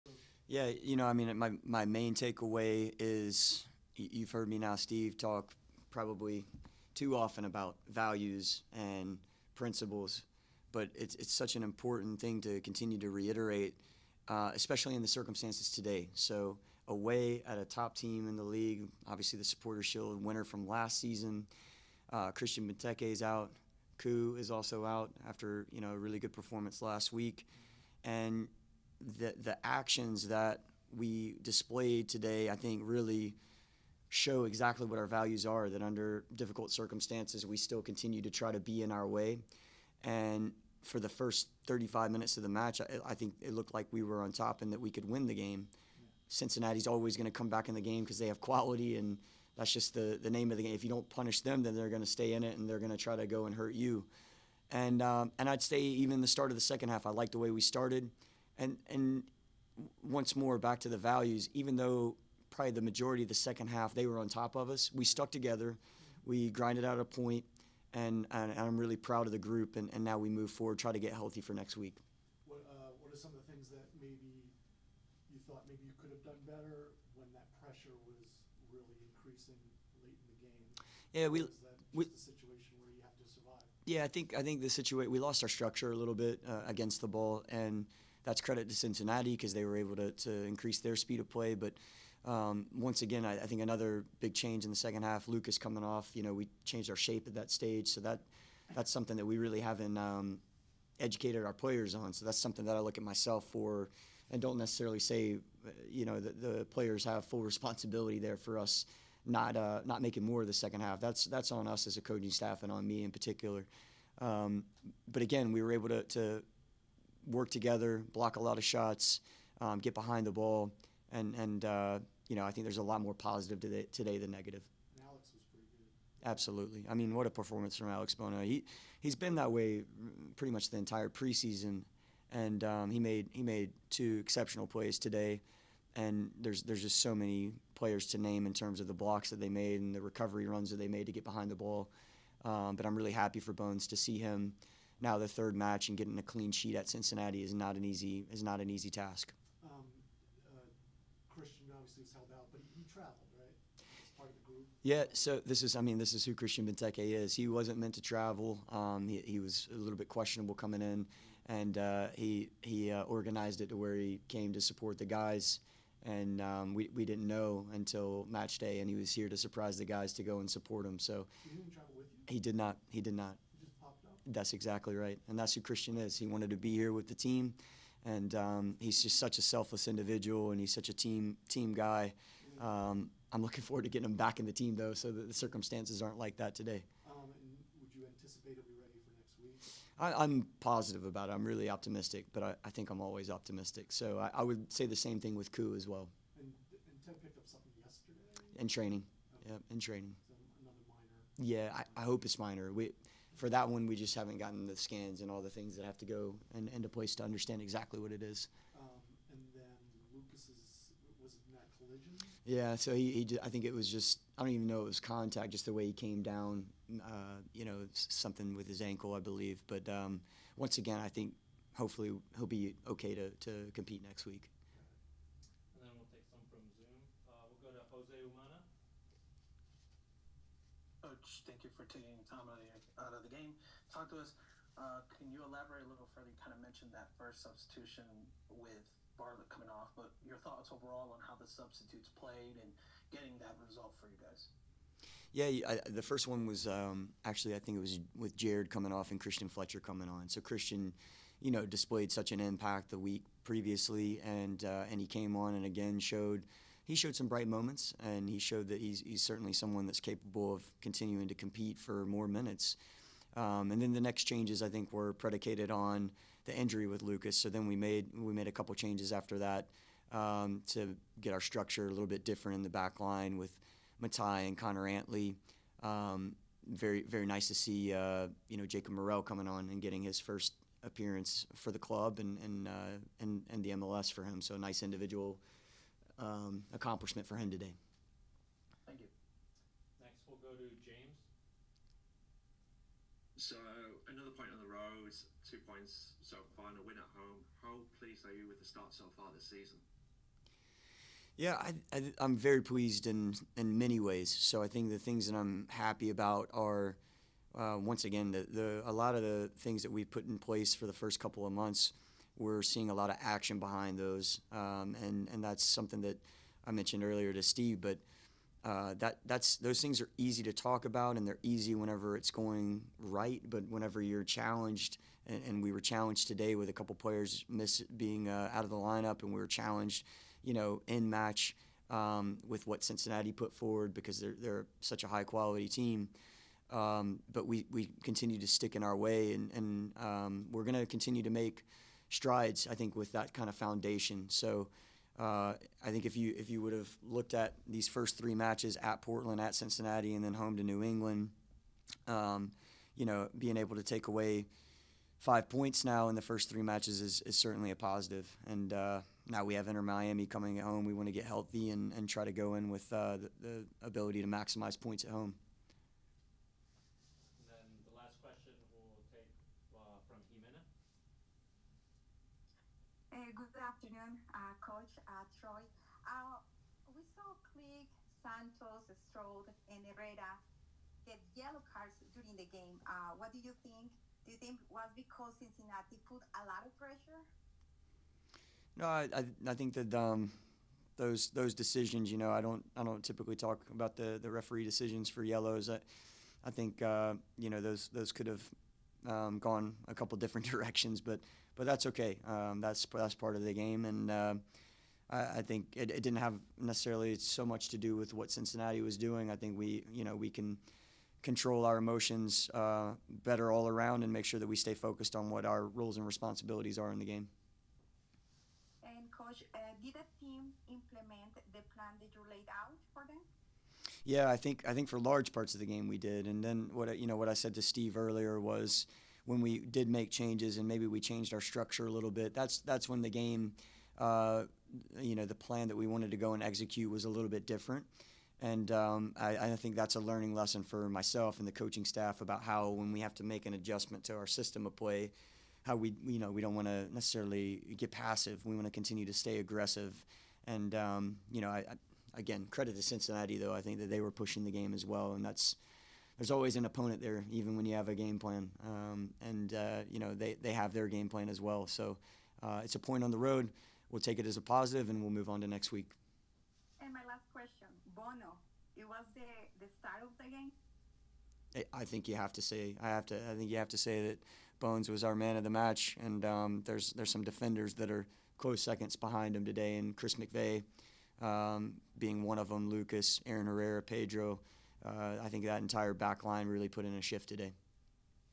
Troy Lesesne Post-Match Press Conference | FC Cincinnati vs. D.C. United | MLS 2024
Troy Lesesne reacts to D.C. United's 0-0 draw against FC Cincinnati.

💻 For all things D.C. United » dcunited.com